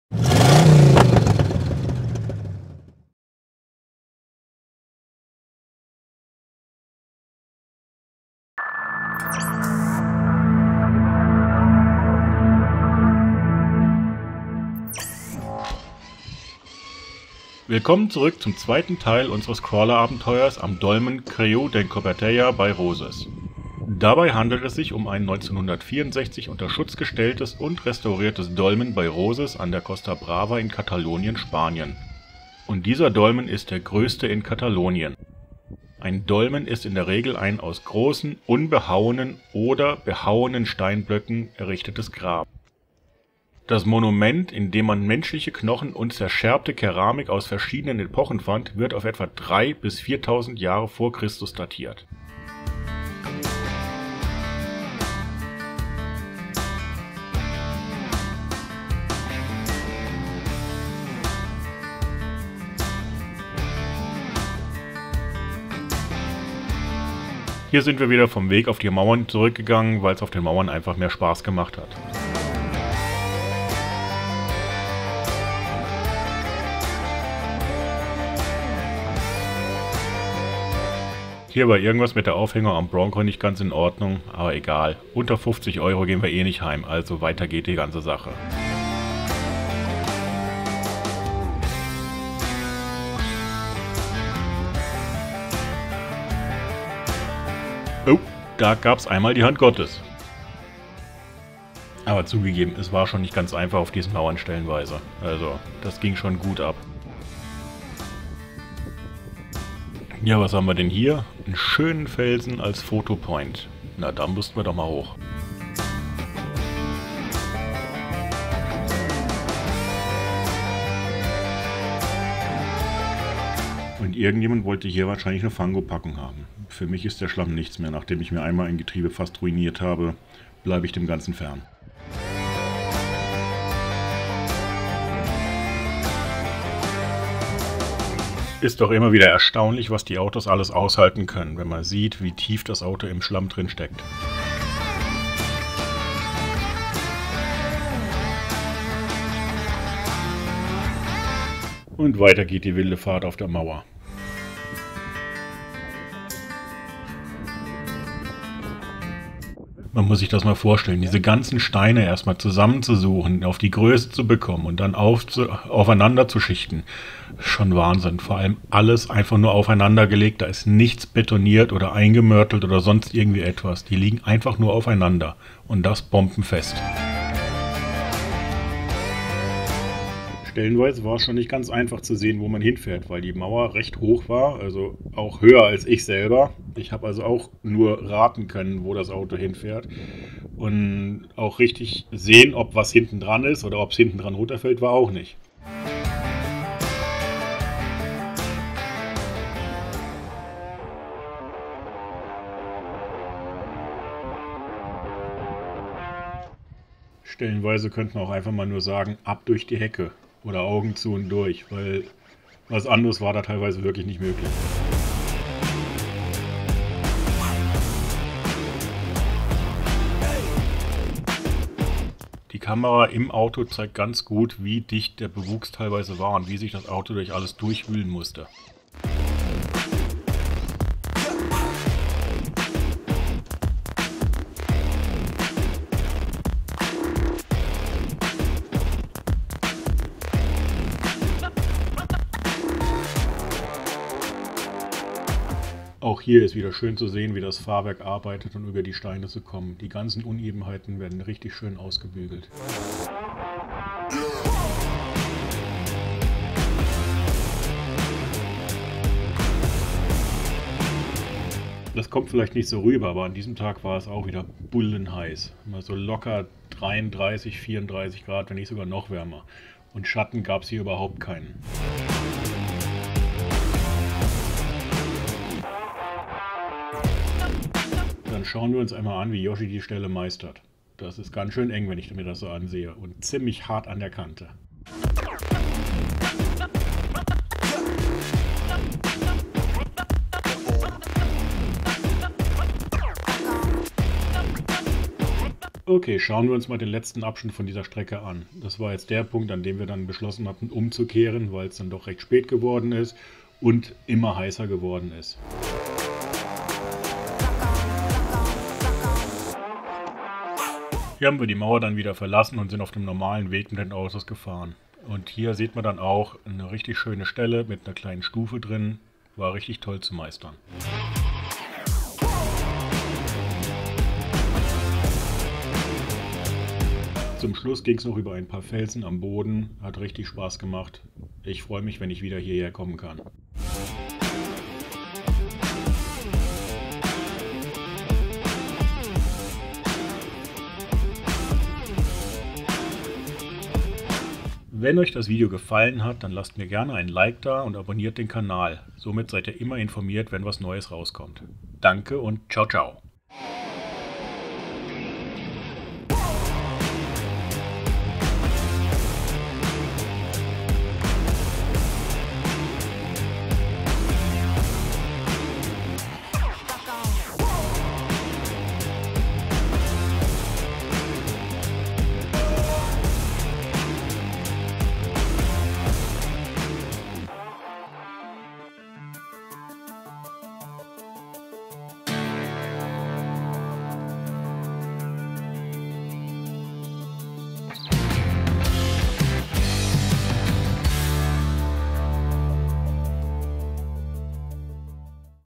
TRAXXAS TRX4 - #49 - Creu d'en Cobertella - Ep. 2
Teil 2 meines Crawlerausfluges zusammen mit Joschi in der Nähe von Roses, Spanien.